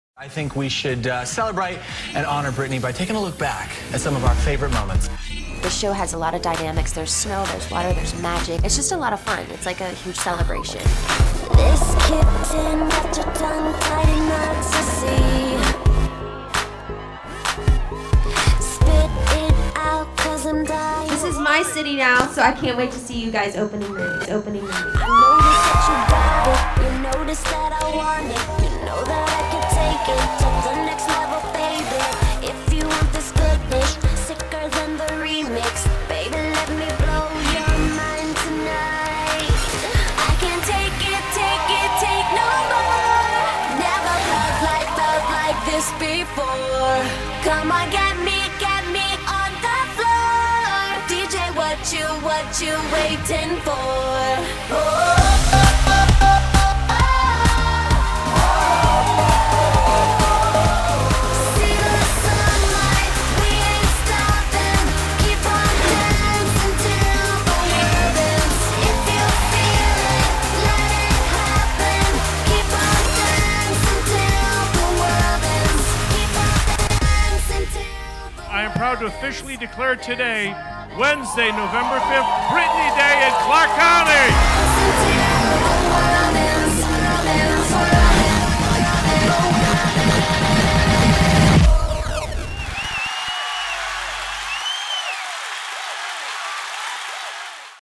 Britney: Piece of Me One Year Anniversary
Britney Spears performing Britney: Piece of Me One Year Anniversary. (C) 2014 RCA Records, The AXIS at Planet Hollywood Resort & Casino, a division of Sony Music Entertainment.